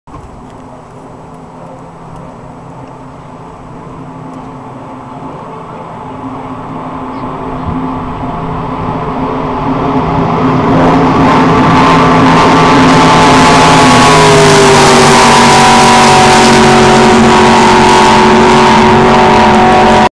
DOIN' DOUGH - 2013 Southern 80 Water Ski Race - 6 Litre
Skiers - Kieran Hinck, Mathew McDonald,
Driver - Paul Collard, Observer - Bryan Dorrington.
2013 Club Marine Southern 80 Water Ski Race.